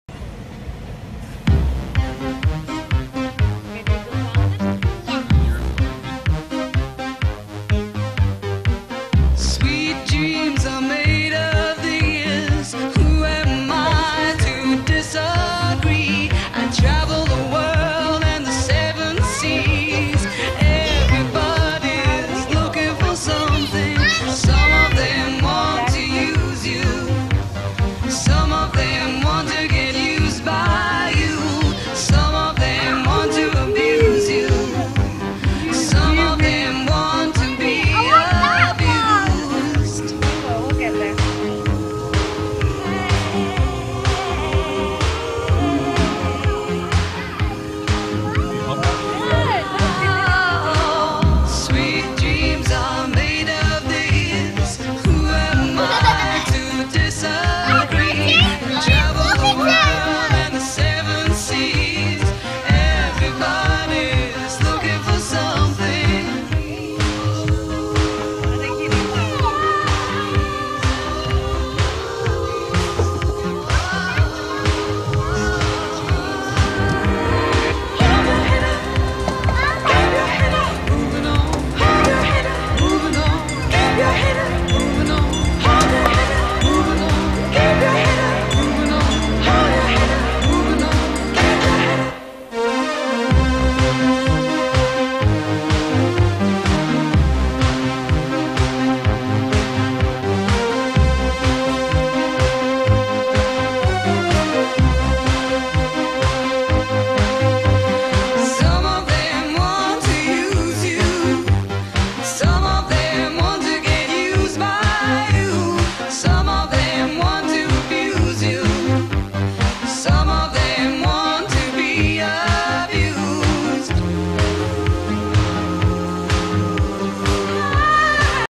Daily vlog. Monster mini golf paramus nj
We took the kids mini golfing. it was a lot of fun. really cute place the kids loved it.